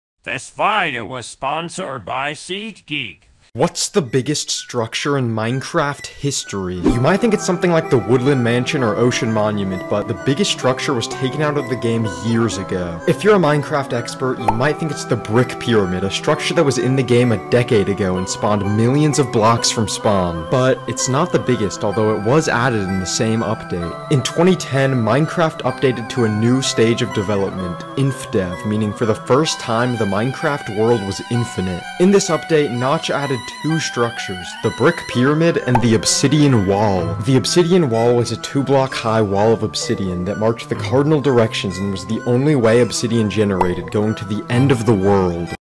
What’s The Biggest Structure In Baldi’s Basics Movie History?
My Very First Sponser Video.
Today We’re Here To Talk About The Biggest Structure In Baldi’s Basics Movie History.

Note: Exepect The Pixelated Version, I Changed I Little Bit Constrainted.